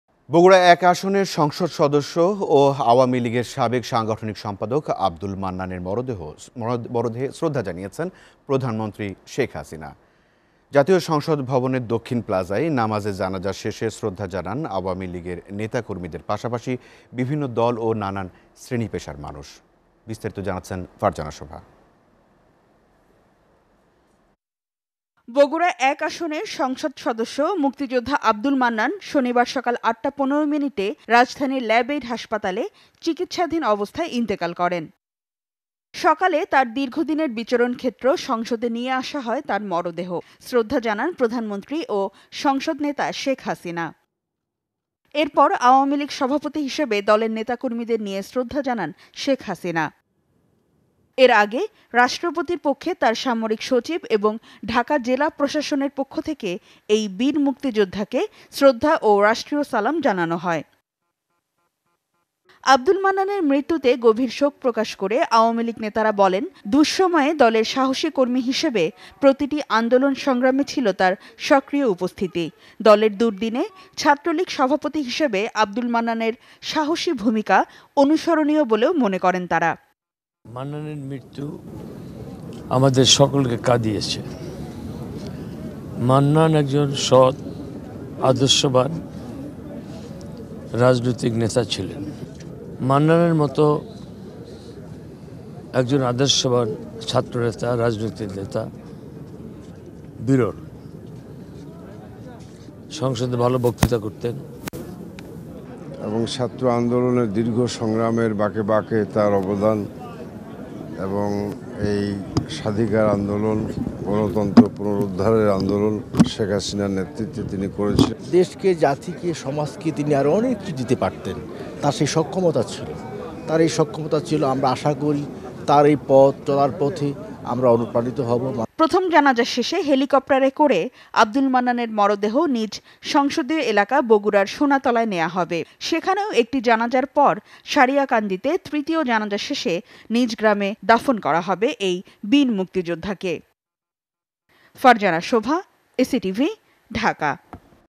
বগুড়া-১ আসনের সংসদ সদস্য আব্দুল মান্নানের মরদেহে শ্রদ্ধা
বগুড়া-১ আসনের সংসদ সদস্য ও আওয়ামী লীগের সাবেক সাংগঠনিক সম্পাদক আব্দুল মান্নানের মরদেহে শ্রদ্ধা জানিয়েছেন প্রধানমন্ত্রী শেখ হাসিনা। জাতীয় সংসদ ভবনের দক্ষিণ প্লাজায় নামাজে জানাজা শেষে শ্রদ্ধা জানান আওয়ামী লীগ নেতাকর্মীদের পাশাপাশি বিভিন্ন দল ও নানা শ্রেনী-পেশার মানুষ।

SA TV Location:
Bangladesh